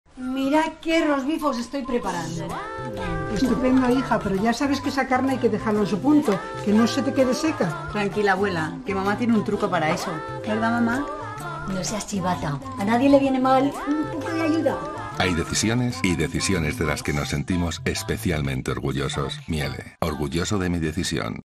La sonda térmica inalámbrica de los hornos Miele
Descubre la sonda térmica inalámbrica de Miele y cómo utilizarla en tus recetas favoritas. ¿Probamos con un rosbif? Pincha aquí para ver la receta completa. Y siéntete orgulloso de tu decisión.

Miele – Siempre mejor. Immer Besser.